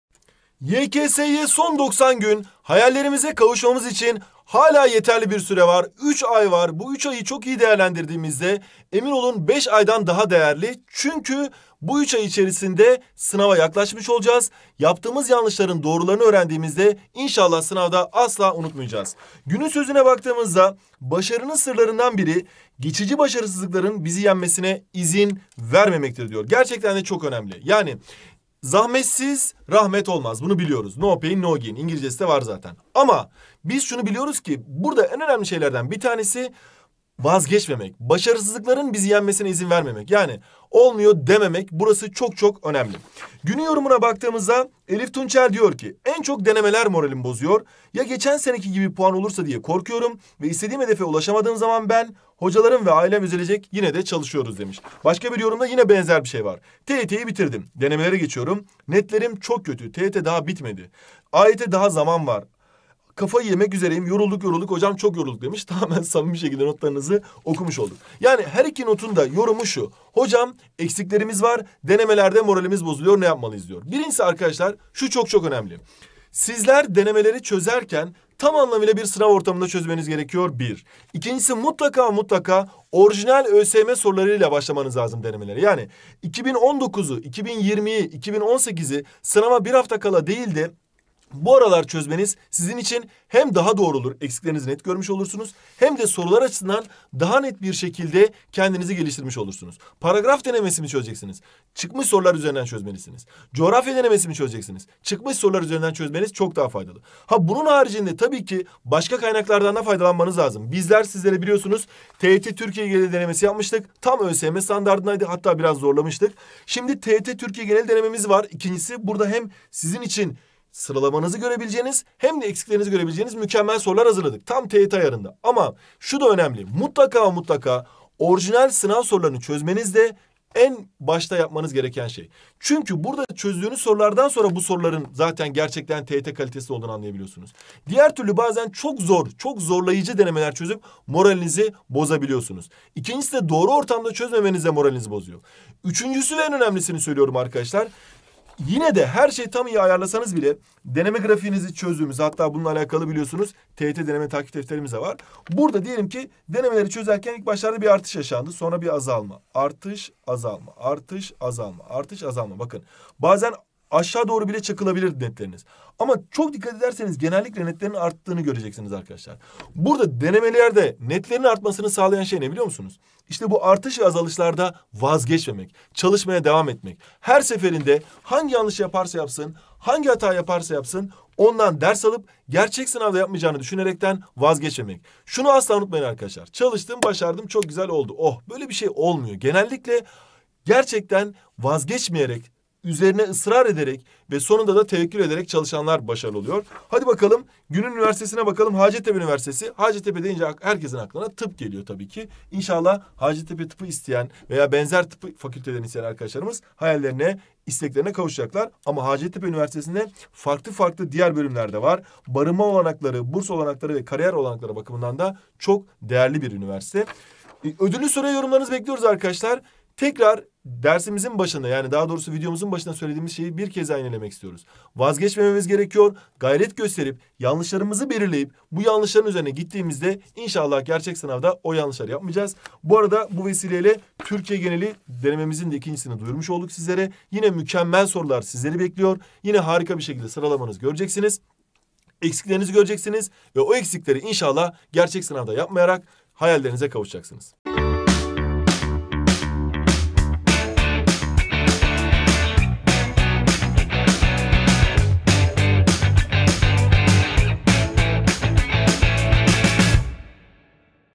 YKS'ye 90 Gün Kaldı! Deneme Sonuçlarında Moralim Bozuluyor! 🤔
YKS'ye 90 Gün Kaldı! Deneme ne sıklıkla çözmelisin? TYT'de konu eksiği olanlar ne yapmalı? Videomuzda anlatıyoruz. 🤓